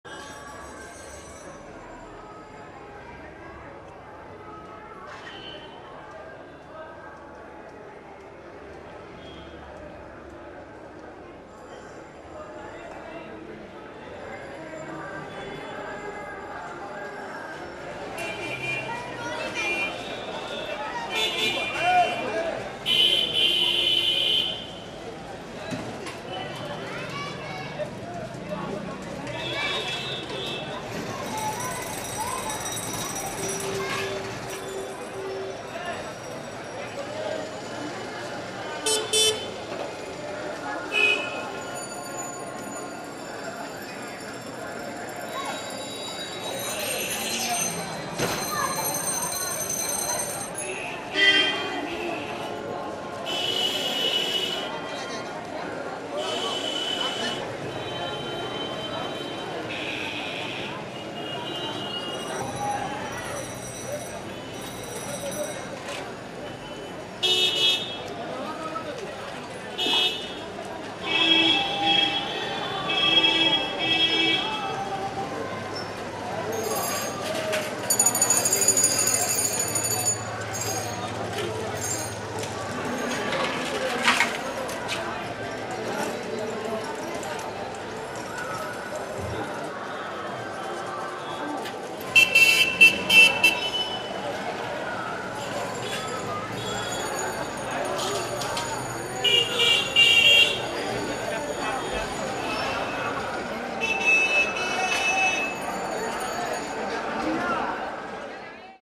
Varanasi. Mattino
Nel dicembre 2012 ho viaggiato in India tra le città di New Delhi, Lucknow e Varanasi.
Questo è il mio primo risveglio a Varanasi. Una scimmietta è venuta a svegliarmi. Prima di uscire sul balcone ho dovuto attendere che se ne andasse. Queste sono le immagini che ho ripreso da quel balcone, un fiume di gente, rumori e colori.
Piergiorgio Mariniello